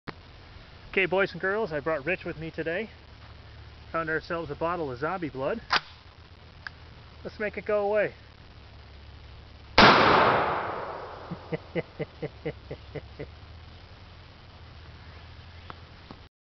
Zombie Blood.AVI
Found me a jug of "Zombie Blood", and just had to shoot it